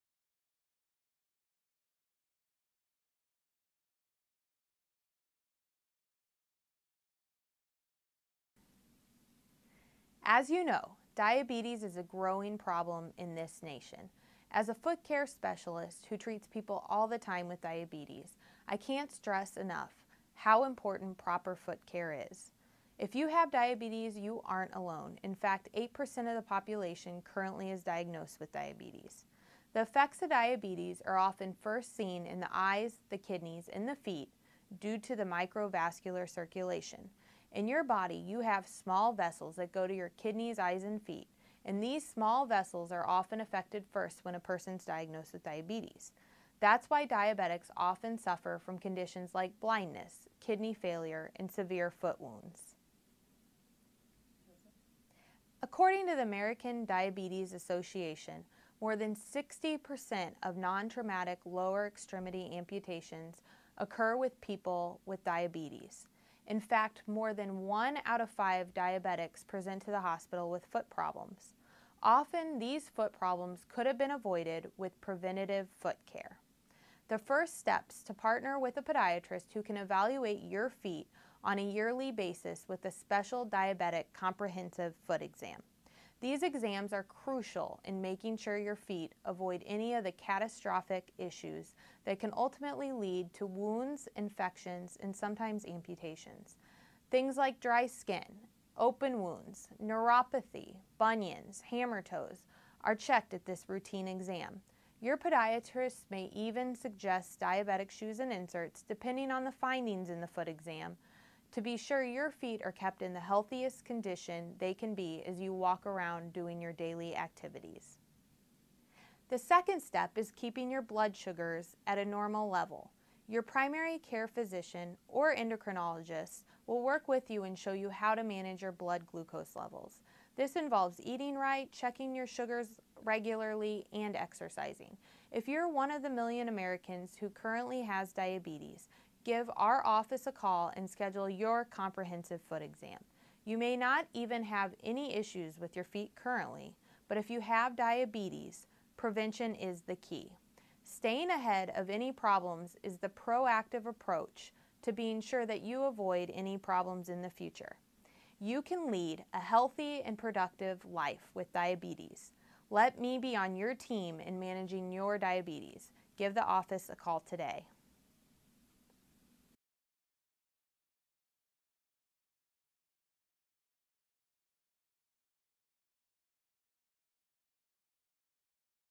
Diabetes and Your Feet
Dr. Stephanie Jameson, podiatrist with Kansas City Foot and Ankle and diabetic wound care specialist, explains why yearly diabetic foot exams are so important.